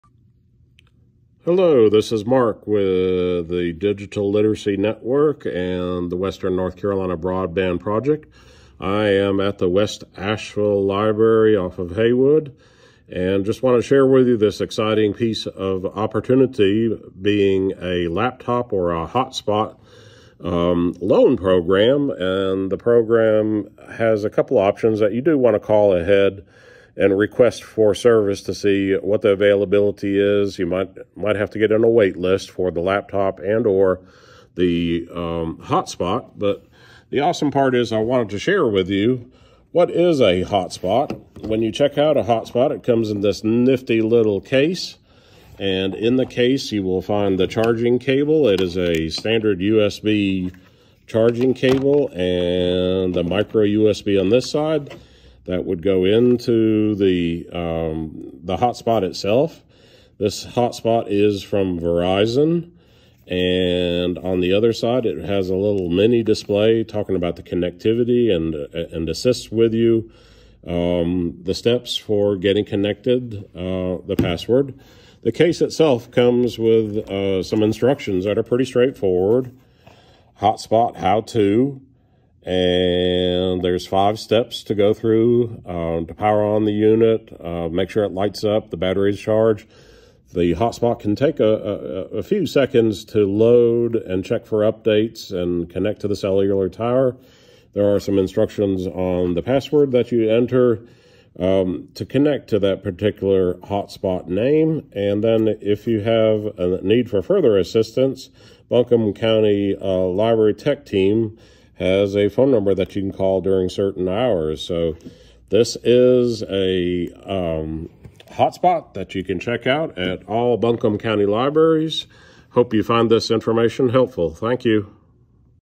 Buncombe County Library Laptop & Hotspot Loan Program
All Buncombe County North Carolina library‘s offer a laptop hotspot loan program that’s available for all library card members that are in good standing you can check out a laptop or a hotspot for up to 30 days they’ll provide the details of the agreement when you come in if you do want to call your local library or to request a device and then the second you’ll receive a phone call when the device is ready to be picked up there is no cost required for this but it is dependent on the availability of the laptop and the hotspot